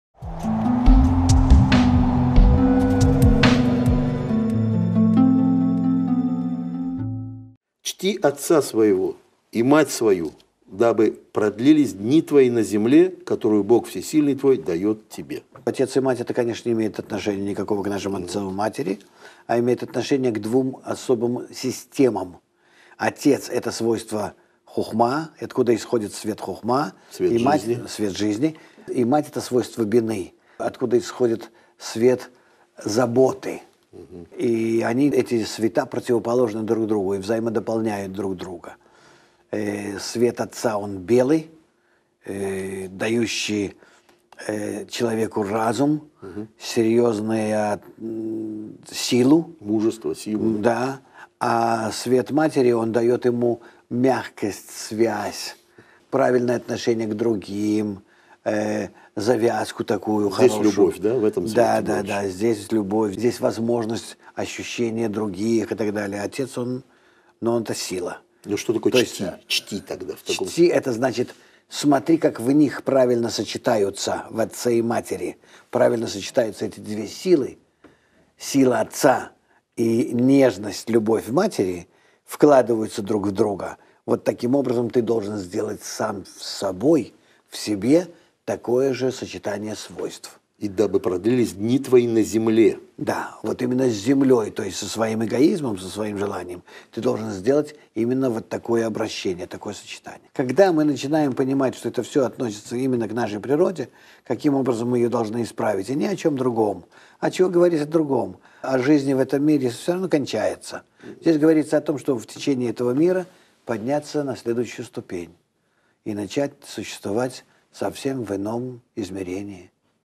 Сила отца и любовь матери. Итро, передача 5
Тора: «Чти отца своего и мать свою, дабы продлились дни твои на земле, которую Бог, Всесильный твой, дает тебе».